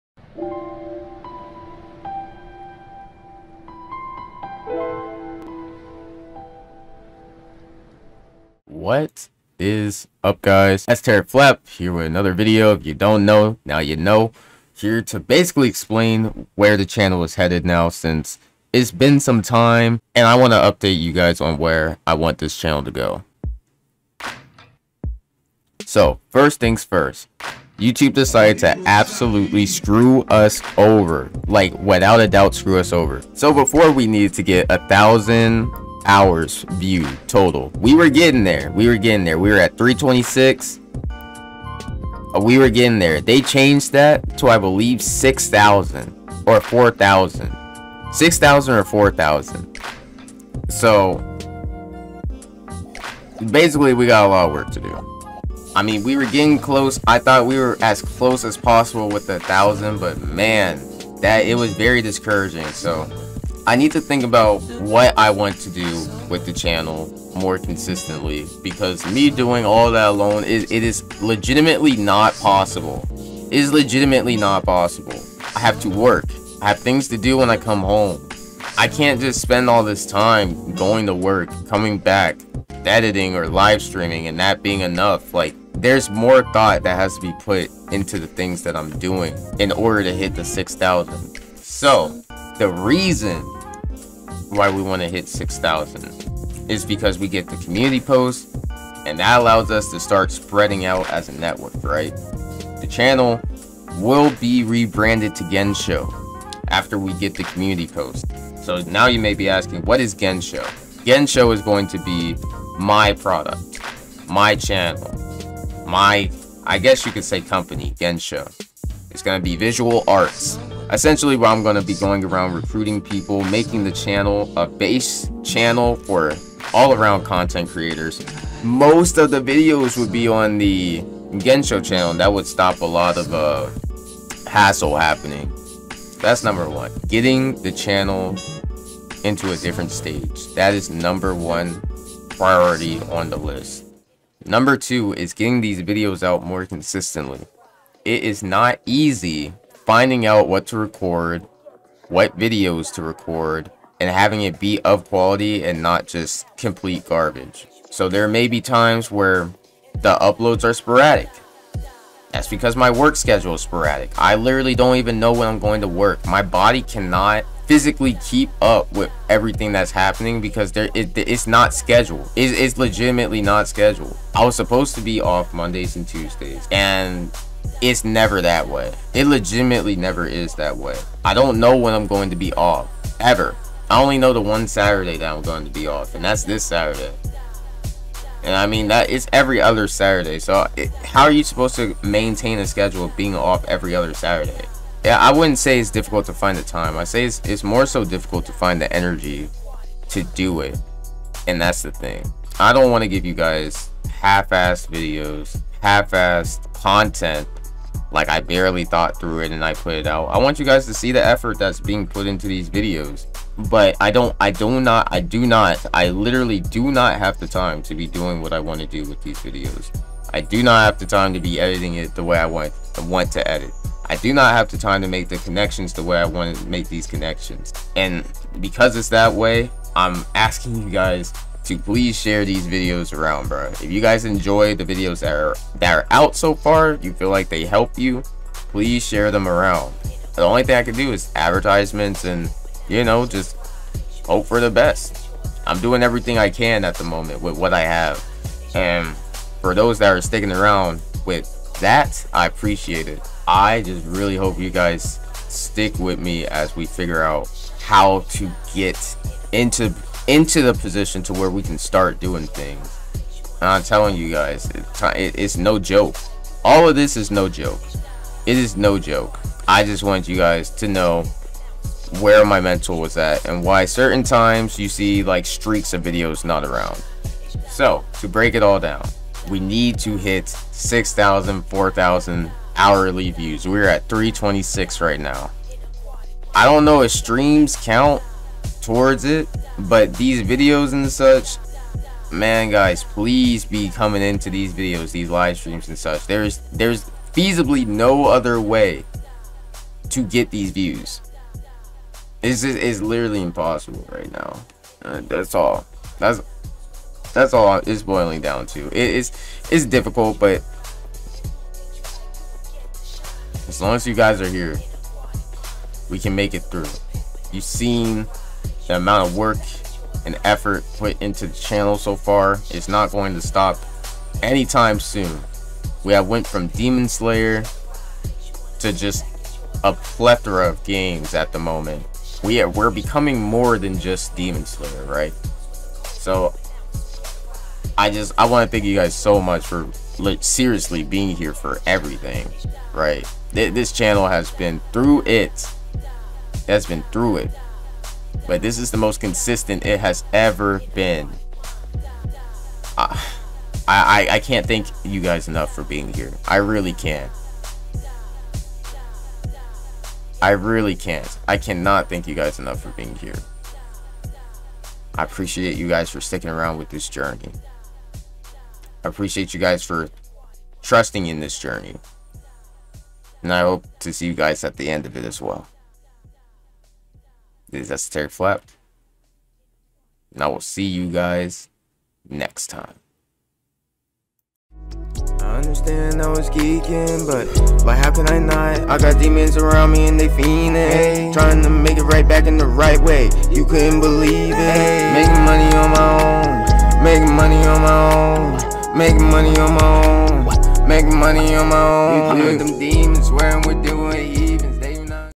The Channel Update - 8/19/2022
This is a more in depth briefing on why I want to do what I do, in the manner it is in.
Chapters:

00:00 - Intro
00:25 - Youtube Changes
01:00 - Thought Process
01:45 - Rebrand
02:23 - Upload Schedule
04:37 - Summary